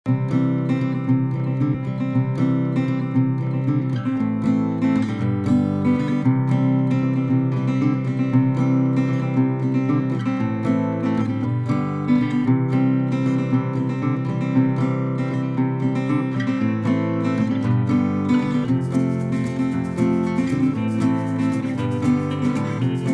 How made the LEGO movie?
A videóban a Választás (LEGO - Love) című filmünk forgatásának láthatjátok néhány pillanatát